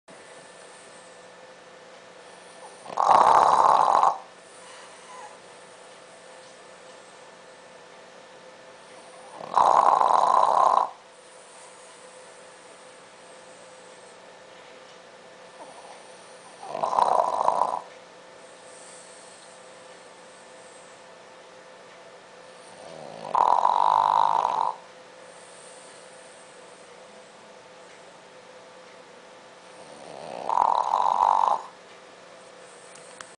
Dexter Snores! - French Bulldog
This is what we sleep with every night.  YES - he MUST sleep with something, usually a bone, in his mouth.